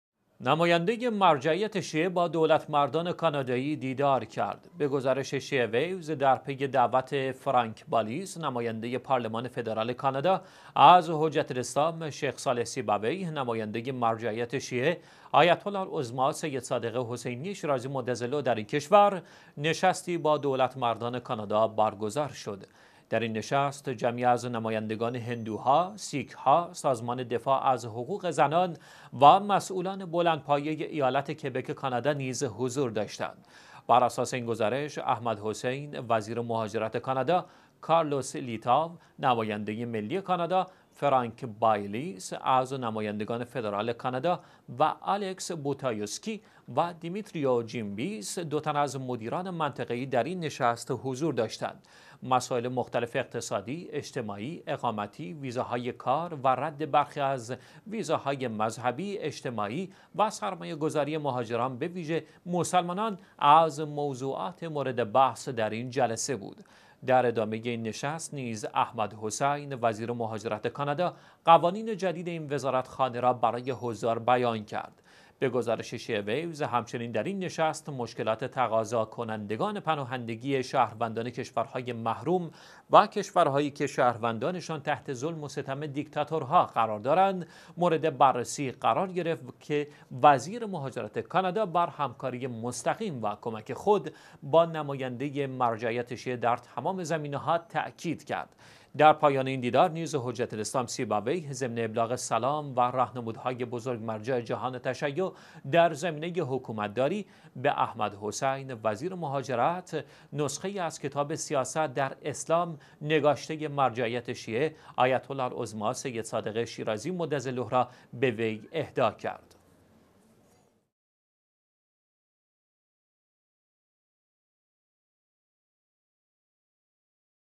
دیدار «حجت الاسلام شیخ صالح سیبویه» نماینده مرجعیت شیعه با دولتمردان کانادا
نماینده مرجعیت شیعه با دولتمردان کانادایی دیدار کرد.
به گزارش شیعه ویوز، در پی دعوت «فرانک باليس» نماینده پارلمان فدرال کانادا، از حجت الاسلام «شیخ صالح سیبویه» نماینده مرجعیت شیعه آیت الله العظمی سید صادق حسینی شیرازی مدظله در این کشور، نشستی با دولتمردان کانادا، برگزار شد.
در این نشست جمعی از نمایندگان هندوها، سیک ها، سازمان دفاع از حقوق زنان، و مسئولان بلندپایه ایالت «کبک» کانادا نیز حضور داشتند.
بر اساس این گزارش ، «احمد حسین» وزیر مهاجرت کانادا، «کارلوس لیتاو» نماینده ملی کانادا،  «فرانک بایلیس» از نمایندگان فدرال کانادا و «آلکس بوتایوسکی» و «دیمیتریو جیم بیس» دو تن از مدیران منطقه ای در این نشست حضور داشتند.
مسائل مختلف اقتصادی، اجتماعی، اقامتی، ویزاهای کار و ردّ برخی ویزاهای مذهبی، اجتماعی و سرمایه گذاری مهاجران ـ به ویژه مسلمانان ـ از موضوعات مورد بحث در این جلسه بود.
در ادامه این نشست نیز، «احمد حسین» وزیر مهاجرت کانادا، قوانین جدید این وزارت خانه را برای حضار بیان کرد.
به گزارش شیعه ویوز، همچنین در این نشست، مشکلات تقاضا کنندگان پناهندگی شهروندان کشورهای محروم و کشورهایی که شهروندان شان تحت ظلم و ستم دیکتاتورها قرار دارند، مورد بررسی قرار گرفت، که وزیر مهاجرت کانادا بر همکاری مستقیم و کمک خود با نماینده مرجعیت شیعه در تمام زمینه ها، تاکید کرد.

در پایان این دیدار نیز، حجت الاسلام سیبویه، ضمن ابلاغ سلام و رهنمودهای بزرگ مرجع جهان تشیع در زمینه حکومت داری، به «احمد حسین» وزیر مهاجرت، نسخه ای از کتاب «سیاست در اسلام» نگاشته مرجعیت شیعه آیت الله العظمی سید صادق شیرازی مدظله را به وی اهدا کرد.